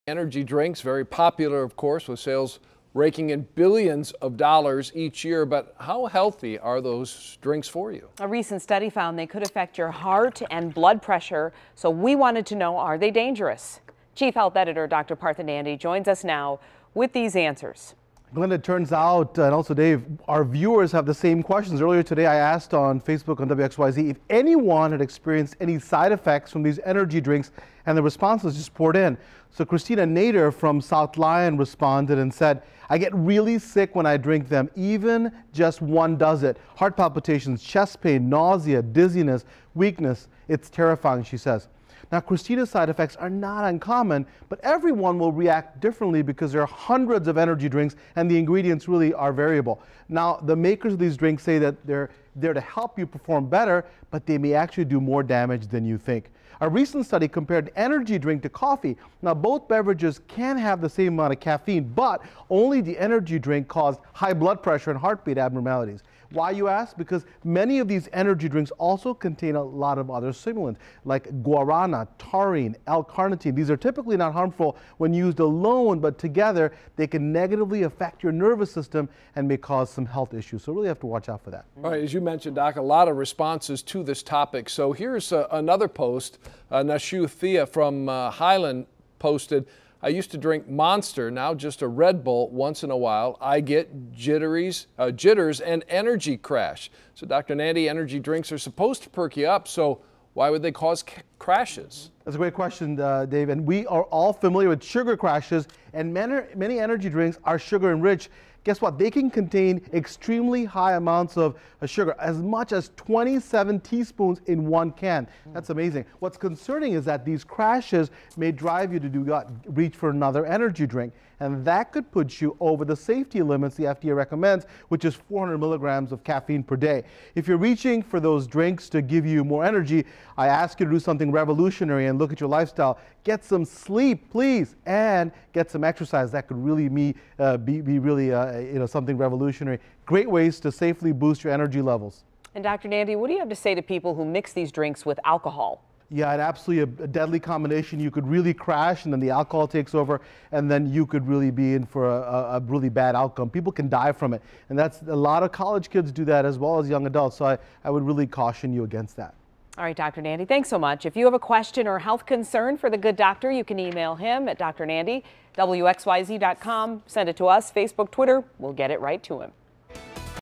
Ask Dr. Nandi: Can energy drinks make your heart beat abnormally?
Energy drinks are very popular with sales raking in billions of dollars - but how healthy are they?   A recent study found they could affect your heart and blood pressure.   I love hearing from our Facebook fans and today I asked them if they’ve experienced any side effects from these energy drinks.